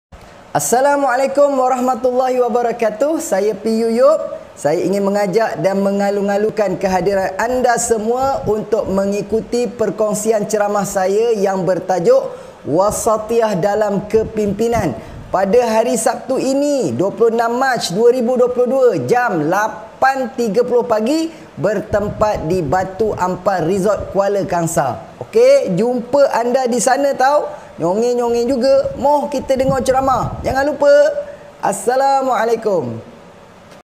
CERAMAH MOTIVASI BERSAMA PU YEOP
Assalamualaikum w.b.t.

Kami dari kepimpinan Kelab Taha@U sesi 21/22 akan mengadakan  ceramah motivasi pada program Kem Transformasi Ummah 7.0 bersama panel jemputan khas dan istimewa iaitu  Pu Yeop.

Berikut adalah ketetapan majlis:

🗓TARIKH : 26 MAC 2022
🌌HARI : SABTU
⏰MASA : 8:30 - 9:30 pagi
📍LOKASI : BATU AMPAR RESORT, KUALA KANGSAR.

In sha Allah dengan menyertai program ini secara tidak langsung dapat
menambahkan lagi ilmu pengetahuan peserta-peserta yang terlibat.